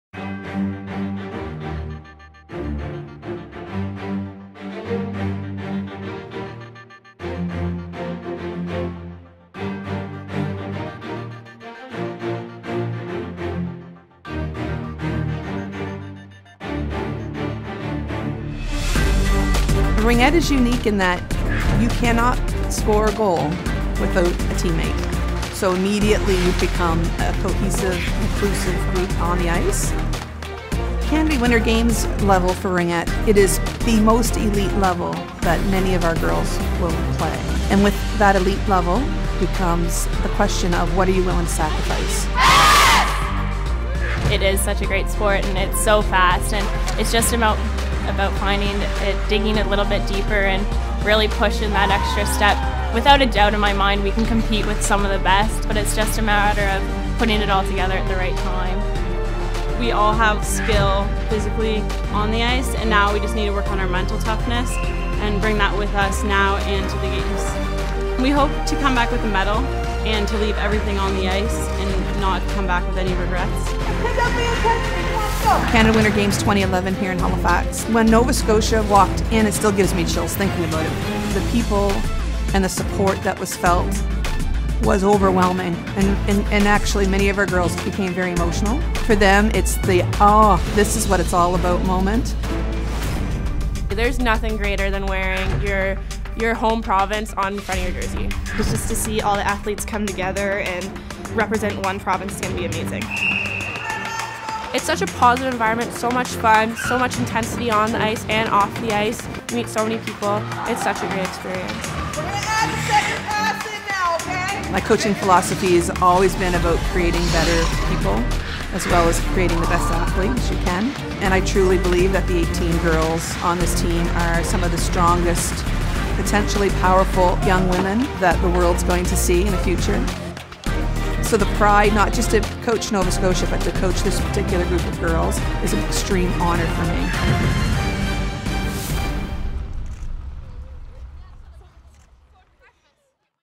Ringette - Team Nova Scotia
The Ringette team will be competing at the 2015 Canada Winter Games, in Prince George, British Columbia.
Play in HD for best quality.